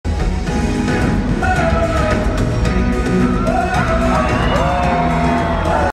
Nalani spit front flip
I wish they would do modifiers with behaviors more often! I love watching them :)
11/5/23